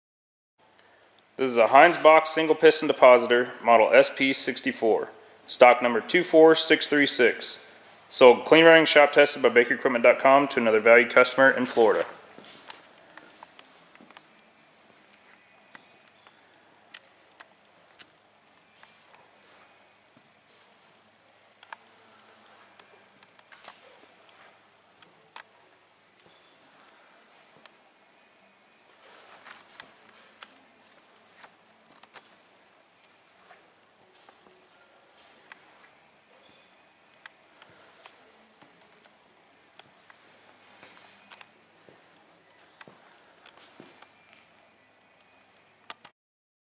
Hinds Bock Single Piston Depositor, Model: SP-64
Stock# 24636
Model: SP-64
Air powered: 4-6 CFM @ 80 PSI
Includes foot controller
O.A. dims: 60" L x 30" W x 64" H